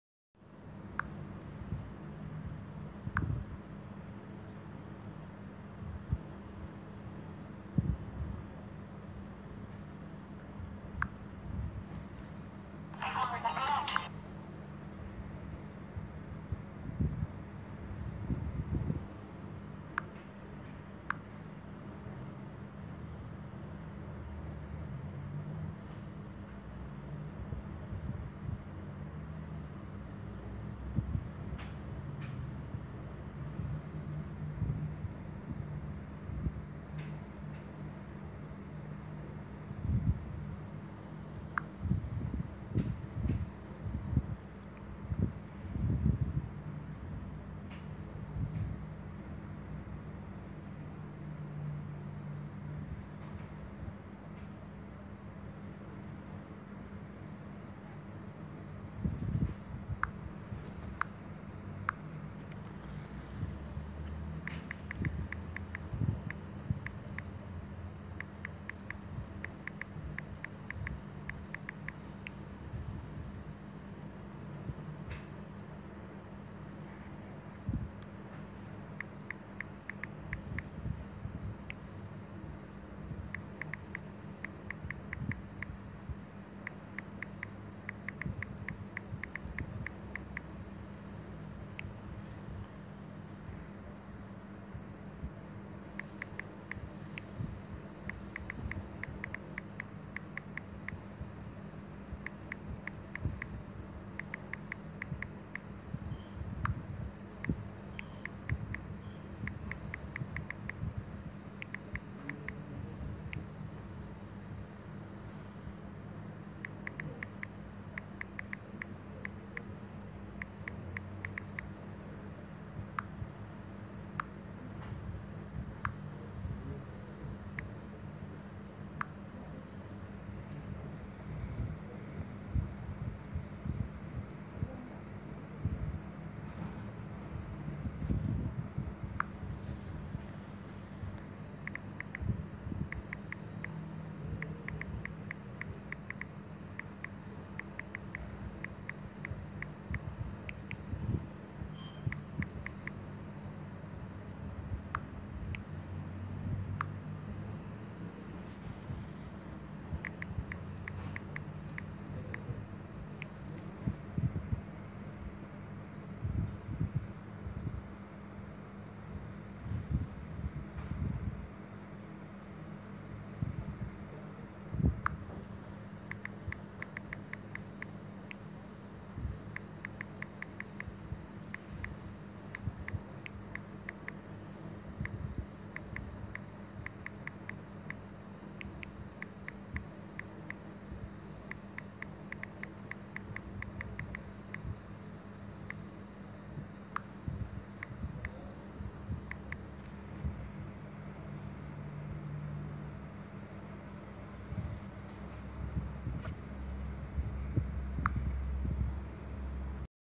Some dude cuss at me and my fans help me
So some dude cuss at my joke video its the karma meme and then all of my supporters help me THANKS YOU GUYS SO MUCH♡♡ LOVE ALL OF MY SUBSCRIBERS//